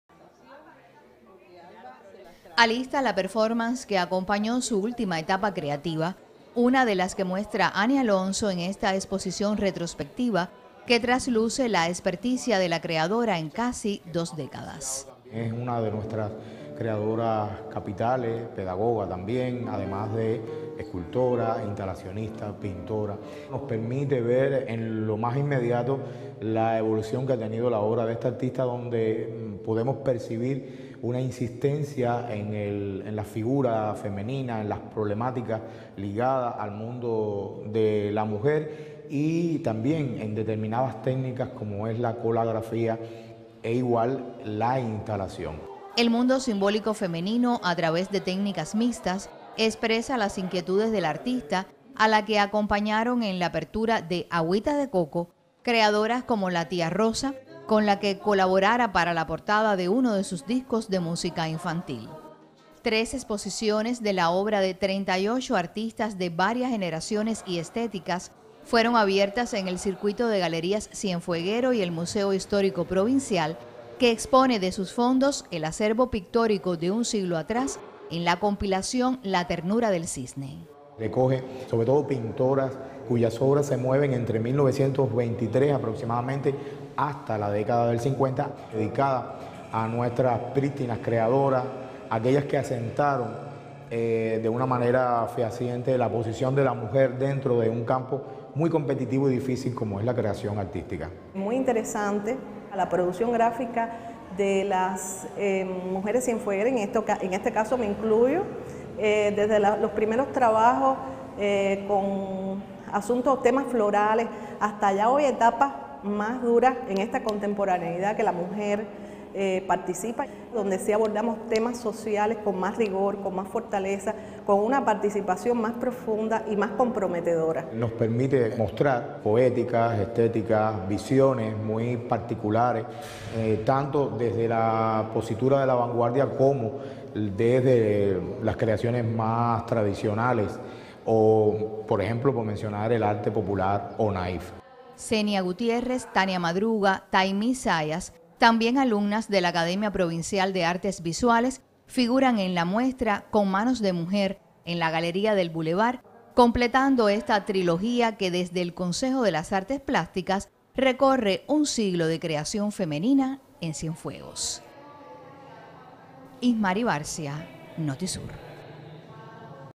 En exposición creación pictórica femenina de Cienfuegos
Tres exposiciones que muestran la obra de más de 38 artistas visuales, fueron abiertas en el circuito de galerías del territorio. Las muestras compilan piezas de un siglo de creación pictórica femenina en Cienfuegos.